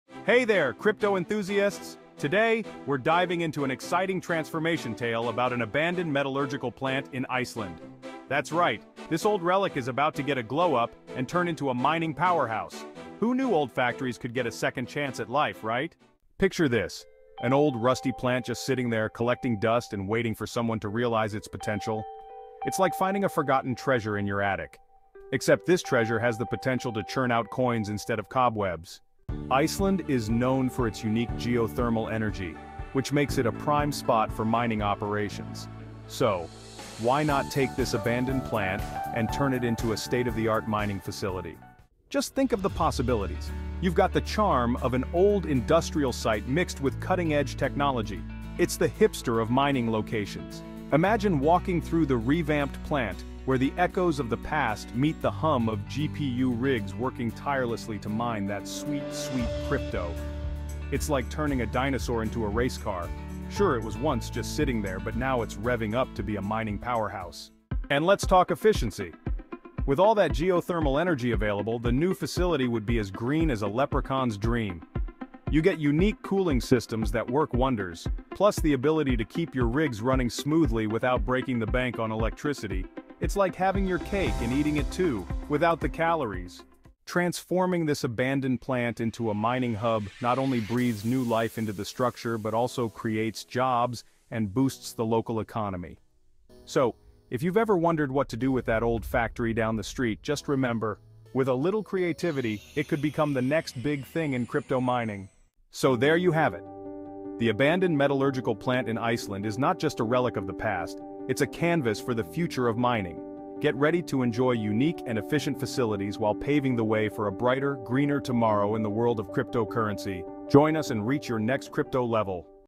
From Ruins to Riches: Transforming Iceland's Abandoned Plant
Join us as we explore the fascinating transformation of an abandoned metallurgical plant in Iceland into a state-of-the-art mining powerhouse!  Discover how harnessing geothermal energy and innovative technology can breathe new life into old structures, creating unique and efficient facilities for the crypto world.

Join CEX.IO Power Tap game to see how you can be part of this exciting journey